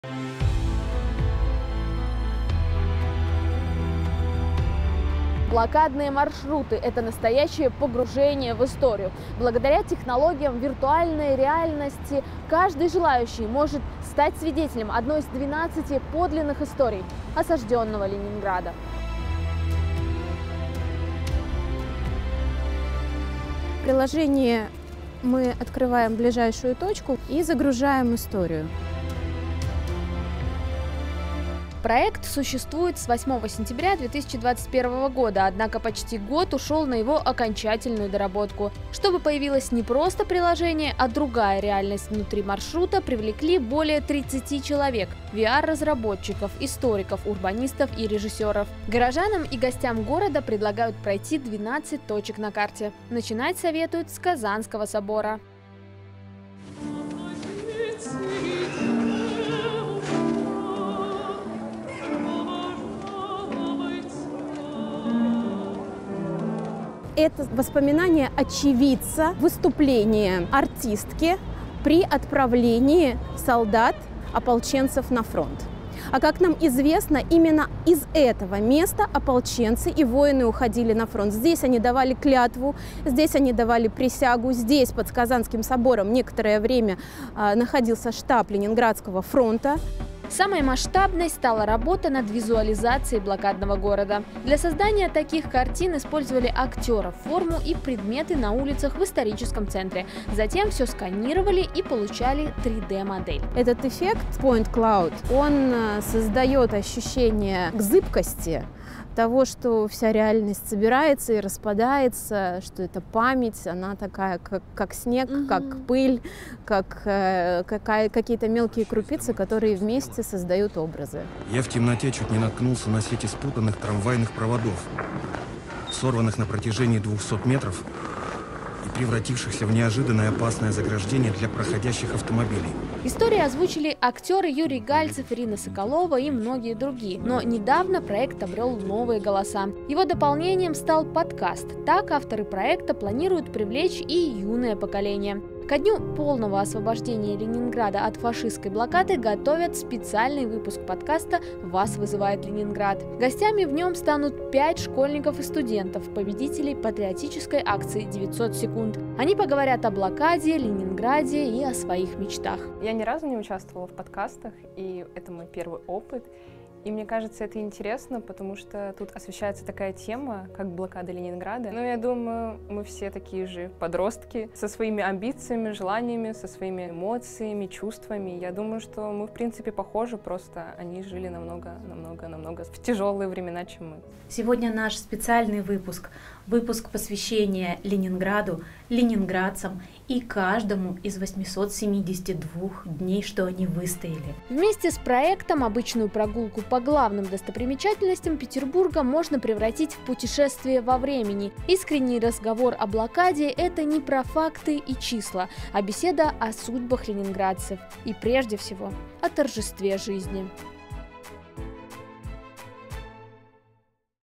«Блокадные маршруты». Прогулка по Ленинграду 1940-х в дополненной реальности
Блокадные маршруты — это настоящее погружение в историю. Благодаря технологии виртуальной реальности каждый желающий может стать свидетелем одной из 12 подлинных историй осаждённого Ленинграда. Анна Смирнова совершила путешествие во времени.